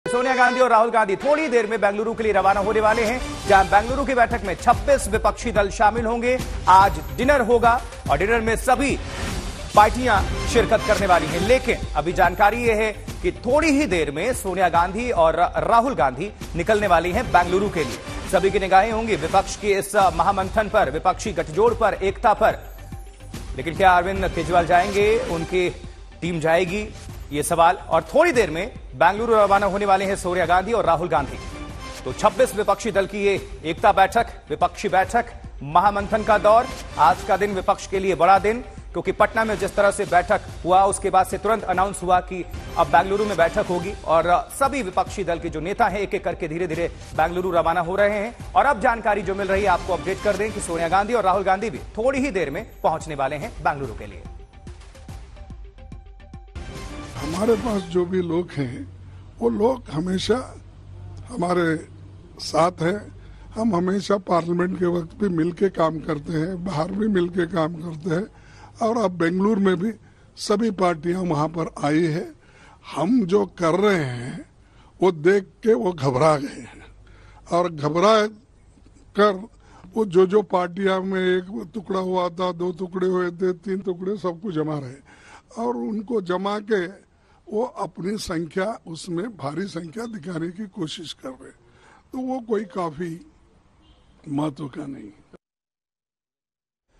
Opposition Meeting: थोड़ी देर में Bengaluru रवाना होंगे राहुल, सोनिया, 2024 के एजेंडे पर होगा मंथन
ब्रेकिंग न्यूज में सबसे पहले बात विपक्ष की महाबैठक की. सोनिया गांधी और राहुल गांधी थोड़ी देर में बेंगलुरू के लिए रवाना होंगे... बेंगलुरु की बैठक में 26 विपक्षी दल शामिल होंगे..विपक्ष की एकता बैठक पर दिल्ली से बीजेपी के सांसद परवेश वर्मा ने जबरदस्त तंज किया है...परवेश वर्मा ने कहा है कि विपक्ष का गठबंधन रावण के 10 चेहरे की तरह है..